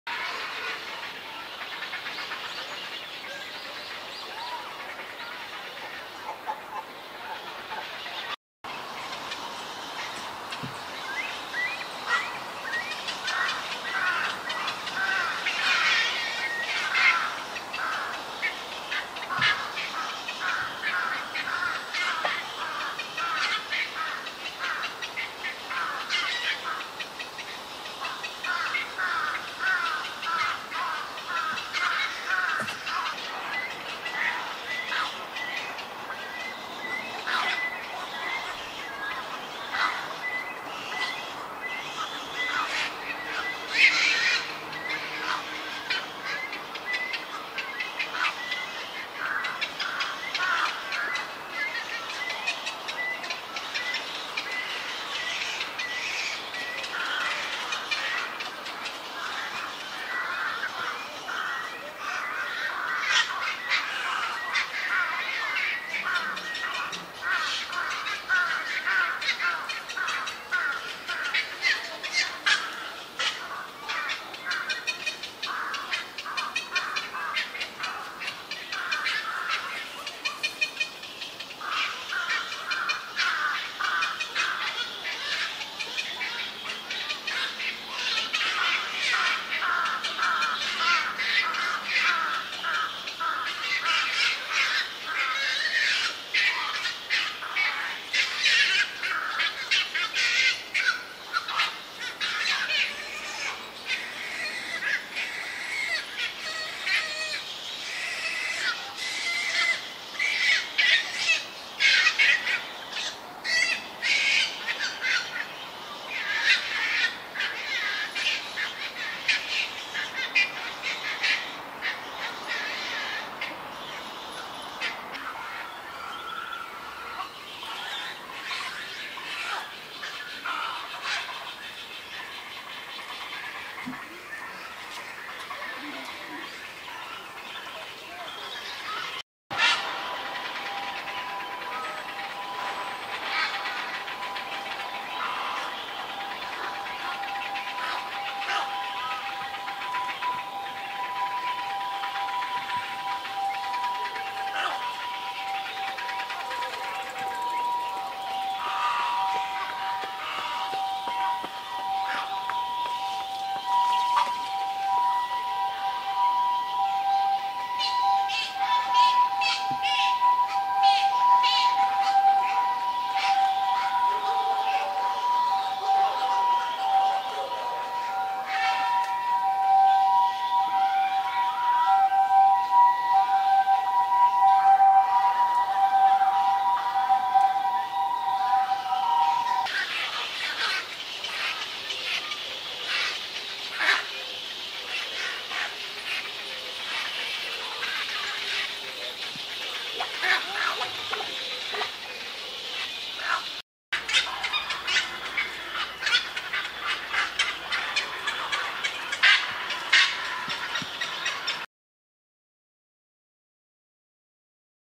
元気一杯！ゴイサギ雛ホシゴイ
２０２３年６月１日　横十間親水公園と仙台堀川公園の交差する野鳥の島
カワウとサギのコロニーになっている。